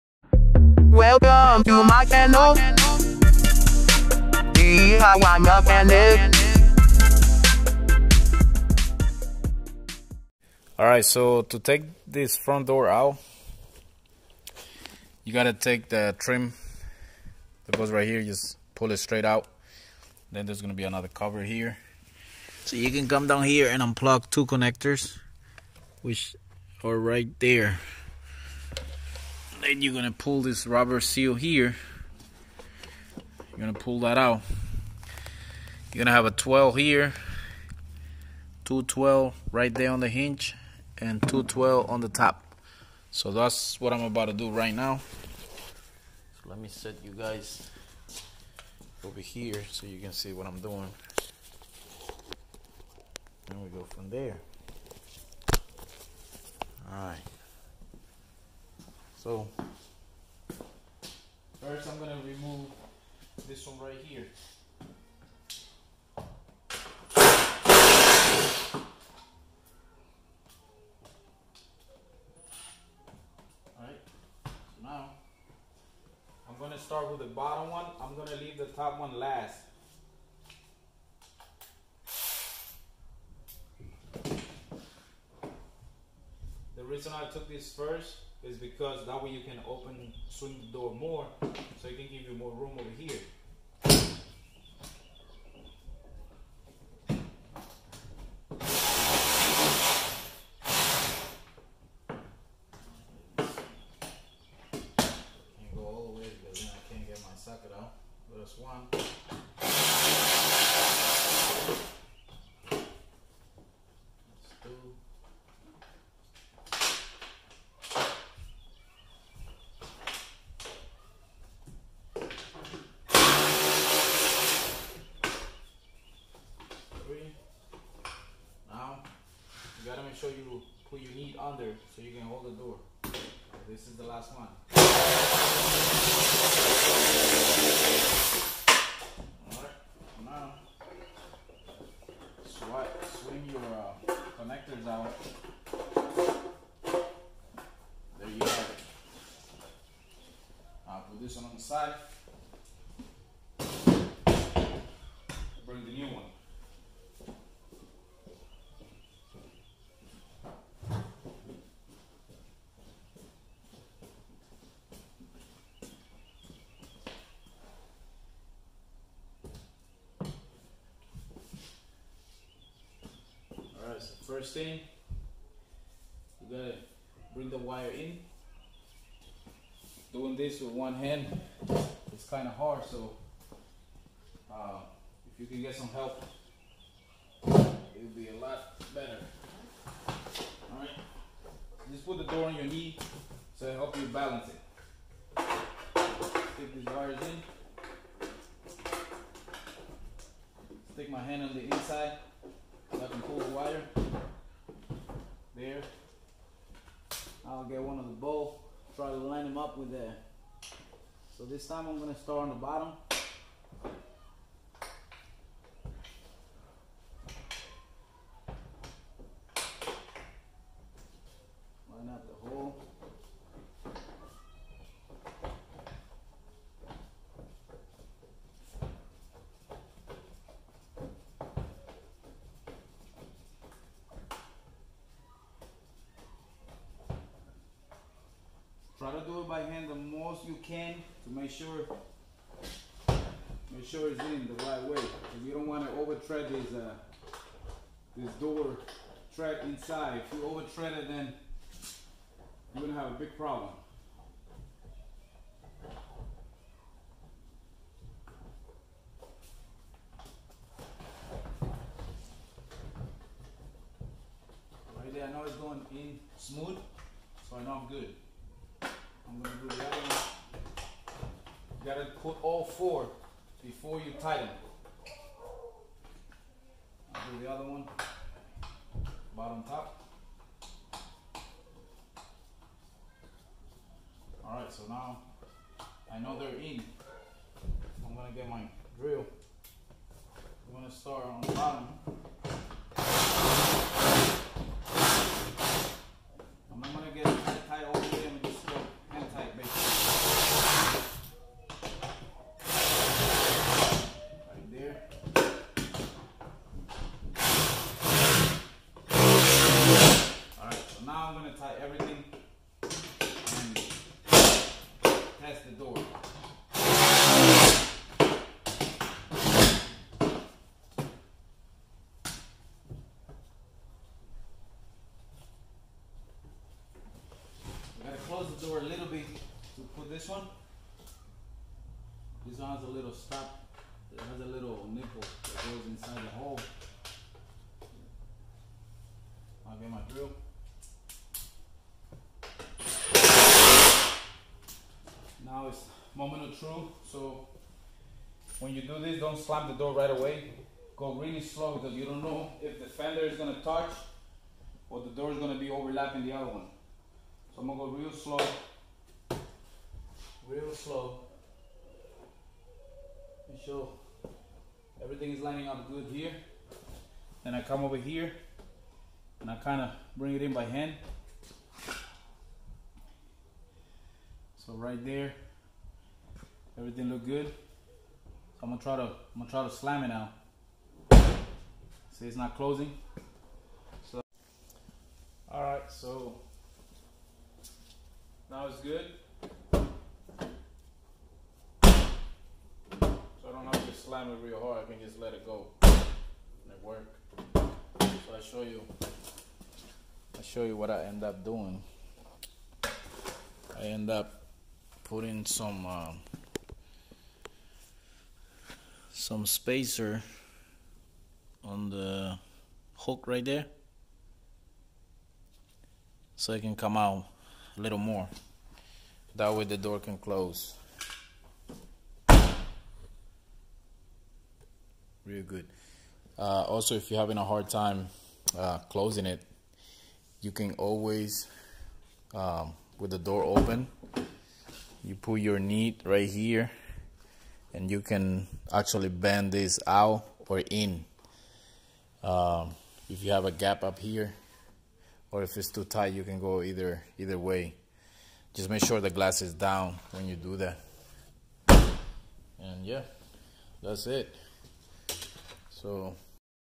DIY Honda civic driver door removal and install
Hi, in this video I will show you how to remove and install your door on your Honda civic!